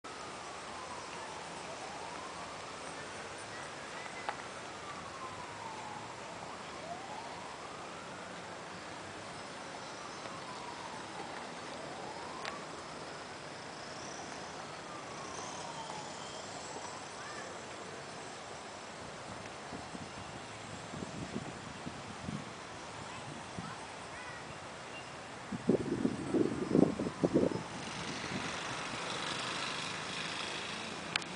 Toyota Rent a Car staff testing the new Toyota Prado in La Sabana, Costa Rica
Toyota Rent a Car Costa Rica

This video was filmed at the Metropolitan Park "La Sabana in San Jose, Costa Rica, while the staff of Toyota Rent a Car conducted a test drive in the Toyota Land Cruiser Prado 2010.